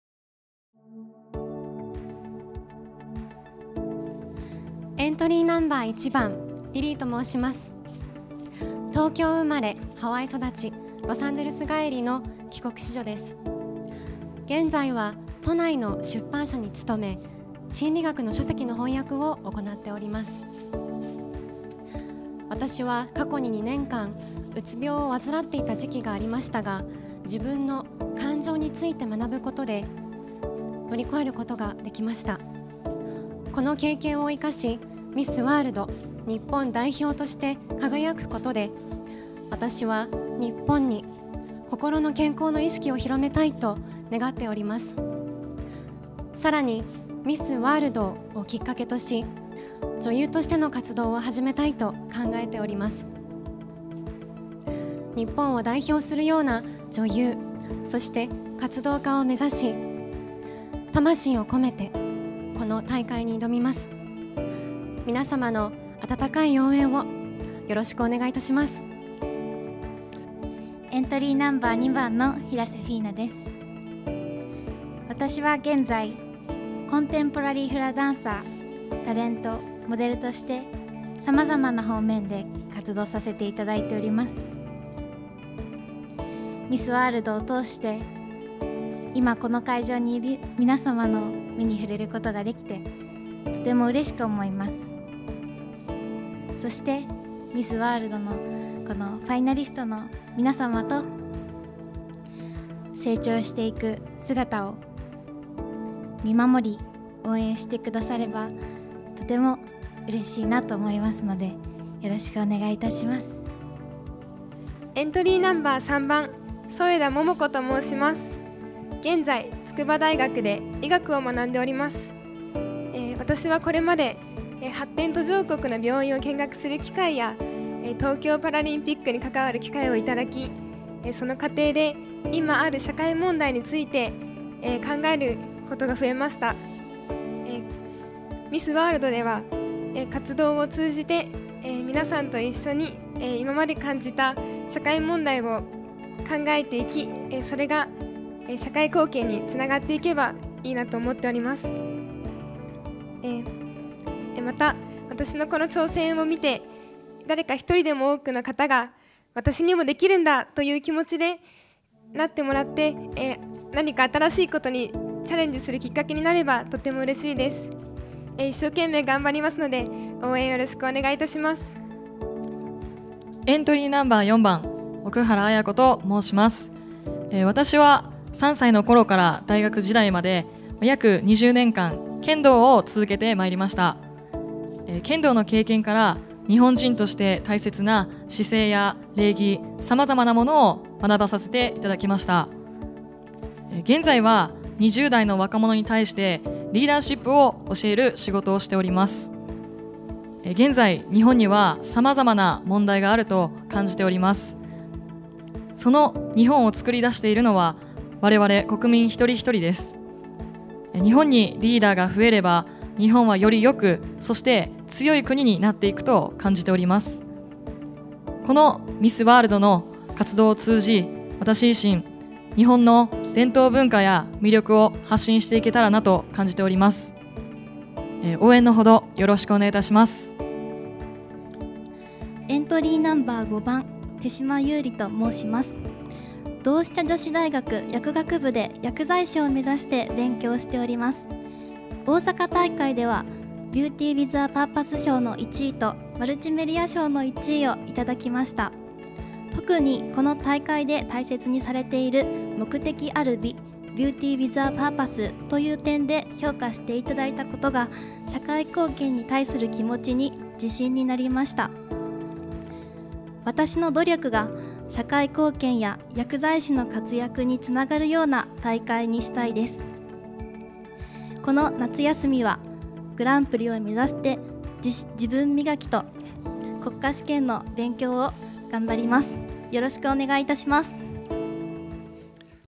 【ミス・ワールド・ジャパン2022】ハワイ育ちの帰国子女、ダンサーなどファイナリスト1～5番
世界大会「ミス・ワールド2022」に出場する日本代表を選出するコンテスト「ミス・ワールド・ジャパン2022」のファイナリスト33人がこのほど、東京都内で開催されたイベントで発表された。会場には32人が集まり、1人は欠席した。

　「ミス・ワールド」は、美に集まる力を社会貢献に生かすことを目的に1951年にスタート。ミス・ユニバース、ミス・インターナショナルと並び「世界3大ミスコンテスト」と呼ばれており、今年で開催72回目となる。「ミス・ワールド・ジャパン2022」への応募総数は7107人で、その中からファイナリストが選ばれた。日本代表を決める日本大会は9月22日にヒューリックホール東京（東京都千代田区）で行われる。